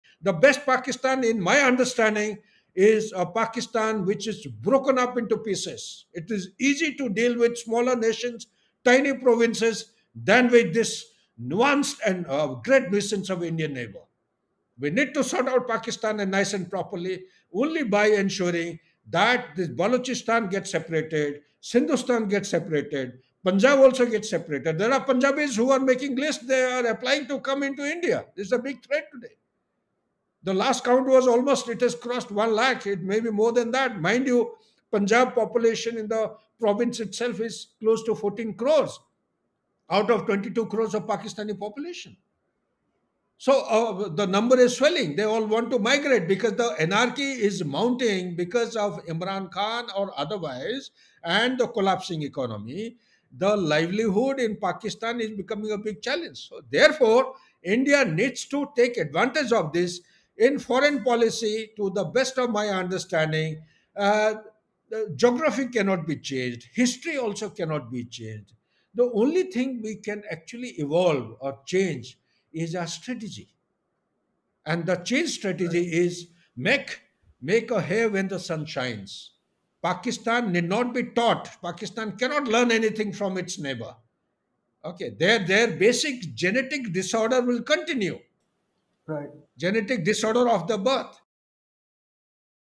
"Even Sindhis, Punjabis In Pakistan Are Applying For Indian Visa.." l Maj Gen Sudhakar Jee (Retd)
Earlier last year, placards of Prime Minister Narendra Modi and other world leaders were raised at a pro-freedom rally in Sindh as protesters demanded the creation of a separate nation of Sindhudesh. In the protest rally people raised slogans demanding the creation of a new country of Sindhudesh and also urged world leaders like Prime Minister Narendra Modi to help them achieve the same.

As per reports, the protesters claimed that Sindh was part of the Indus Valley Civilisation and was illegitimately occupied by the British who then delivered it to Pakistan.

Political stability has been on the loose in Pakistan time and again, and this time, the protests in Sindh, Punjab and Balochistan are on the rise. Maj Gen Sudhakar Jee explains the reason behind this phenomenon.